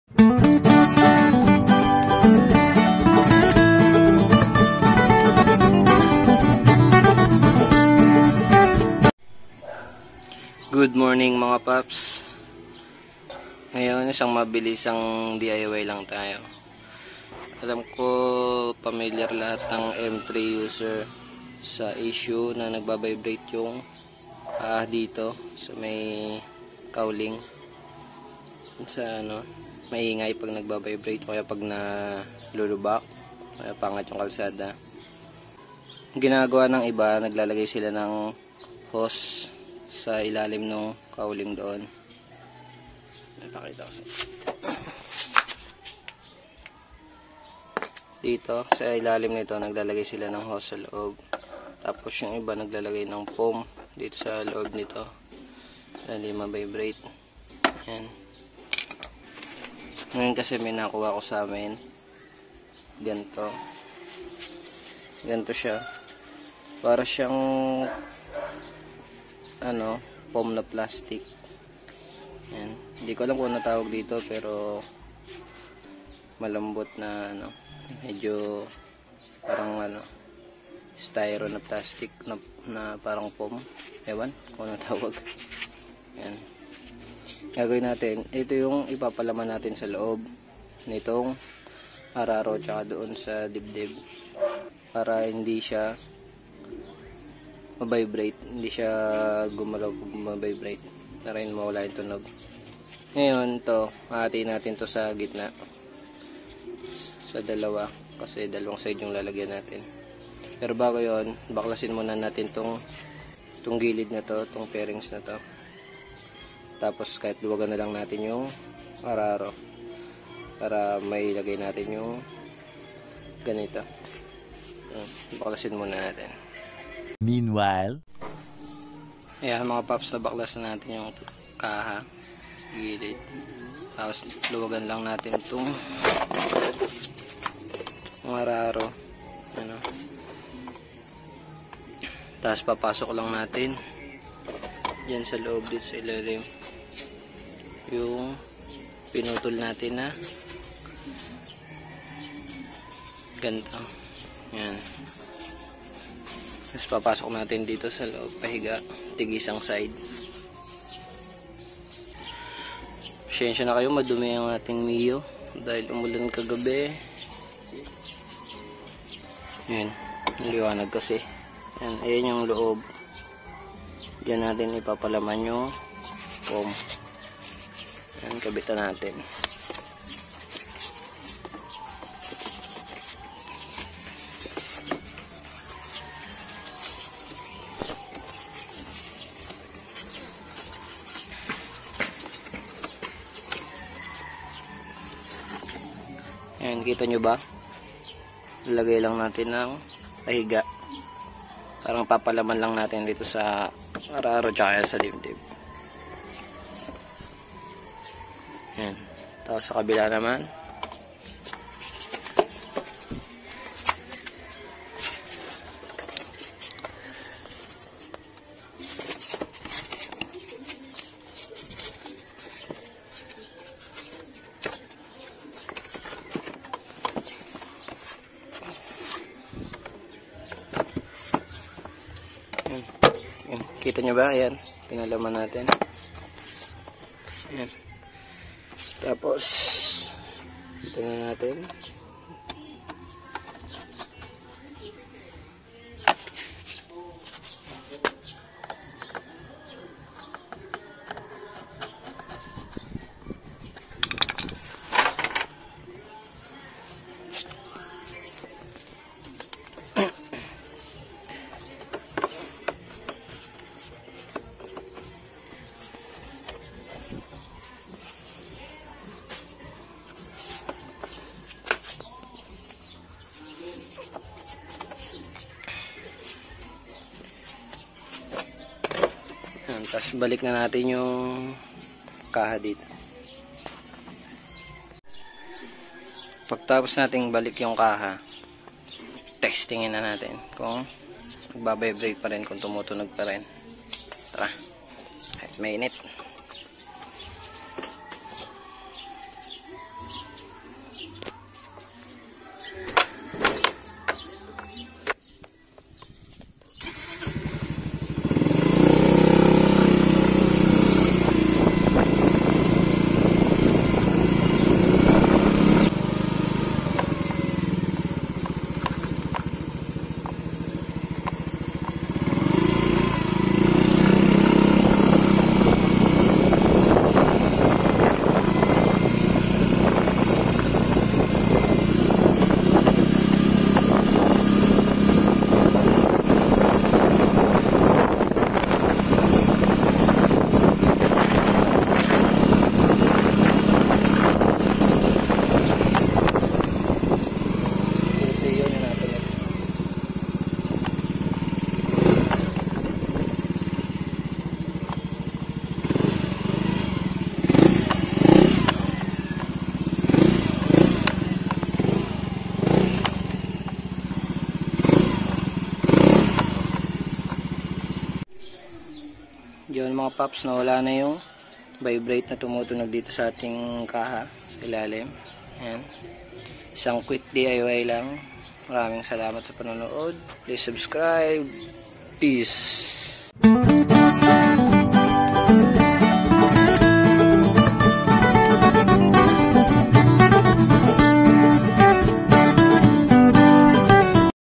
How to remove vibration on mio i125 simple steps
quick DIY, you can use any soft like foam or hose as long as it doesn't make squeaky noise from rubbing/vibrating when place between cowlings. I dunno what's called on that thing I used in the video, I just found it in our home and got an idea to use it as a cushion/stopper.